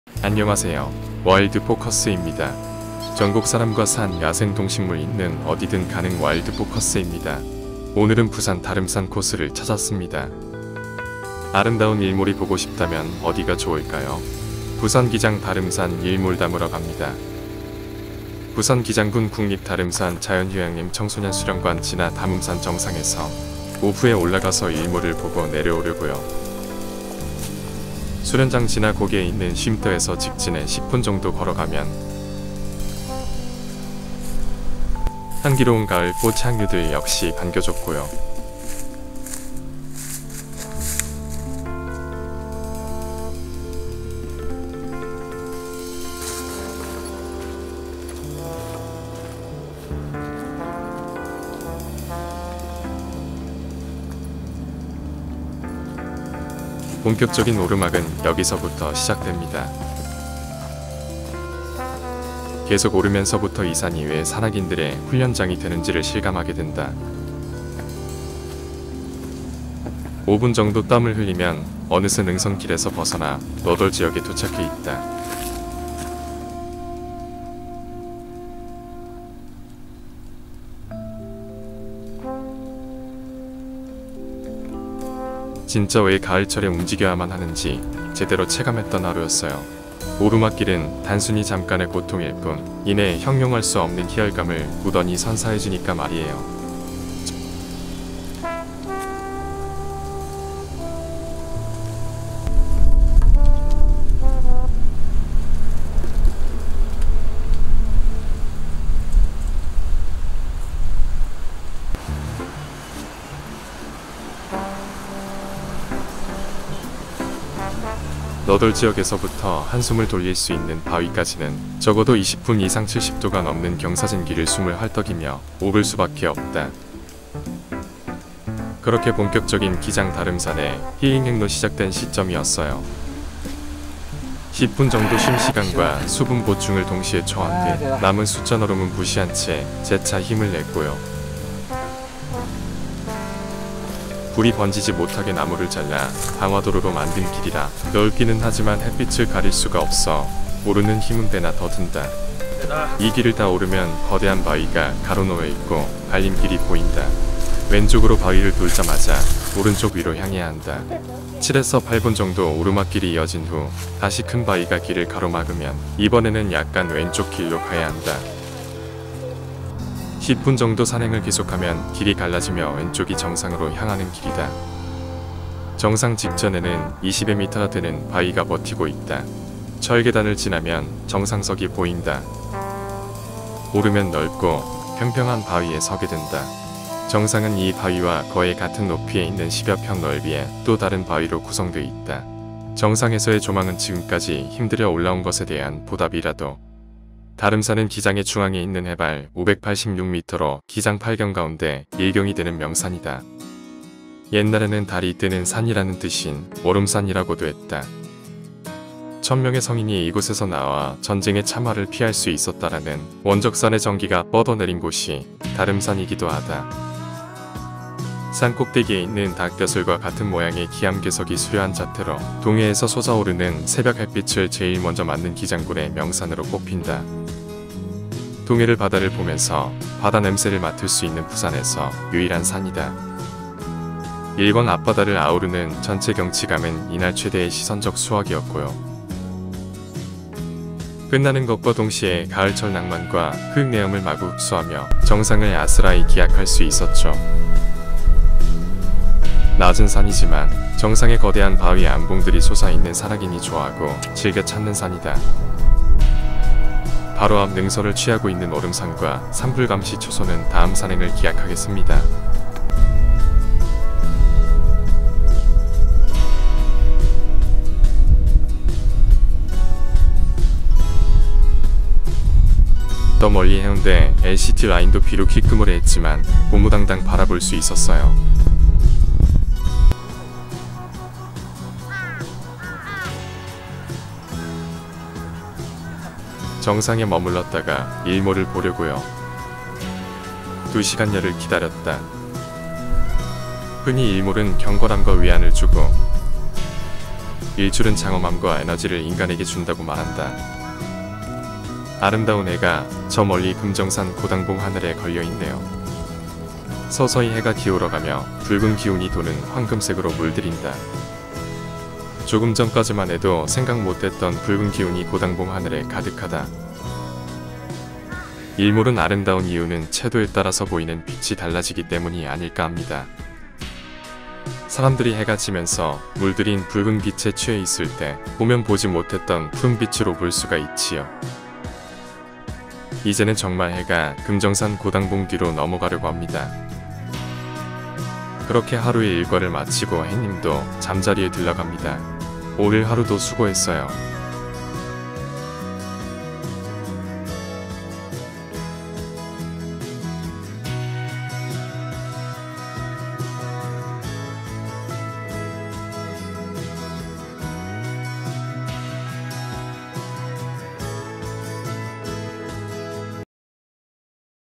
[와일드Focus UHD] 지리산 촌놈, 부산 달음산 가다/저녘노을 이랄까! 일몰 이랄까!
[와일드Focus UHD]

정상에 머물렀다가 일몰을 보려고요. 2시간여를 기다렸다.
아름다운 해가 저 멀리 금정산 고당봉 하늘에 걸려있네요.
서서히 해가 기울어 가며 붉은 기운이 도는 황금색으로 물들인다.
조금 전까지만 해도 생각 못 했던 붉은 기운이 고당봉 하늘에 가득하다.

일몰은 아름다운 이유는 채도에 따라서 보이는 빛이 달라지기 때문이 아닐까 합니다.
사람들이 해가 지면서 물들인 붉은 빛에 취해 있을 때 보면 보지 못했던 푸른빛으로 볼 수가 있지요.